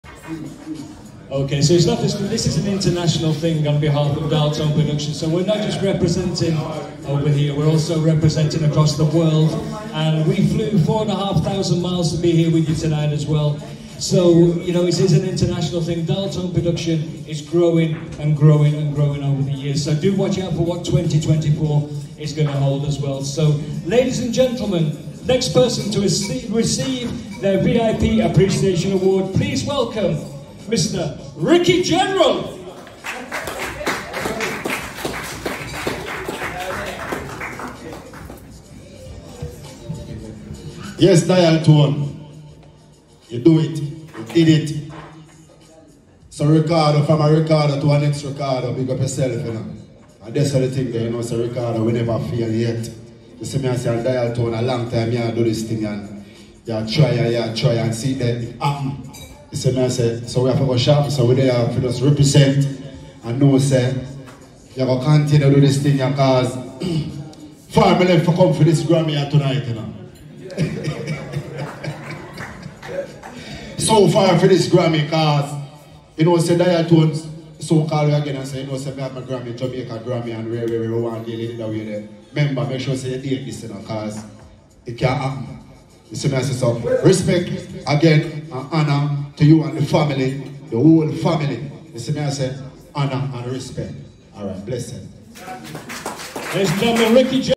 ricky%2520general(1).mp4
🌟 Grateful for the incredible night at Dialtone Productions' Annual VIP Awards Ceremony and Xmas Party! Huge thanks to everyone who came out and supported – your energy made it unforgettable! Shout out to the amazing artists and our Dialtone family. Big up DJ Mani for the music 🔥 big up DJ Delux for hosting 🔥 big up the CEO Ricardo Plummer 👑 Dialtone family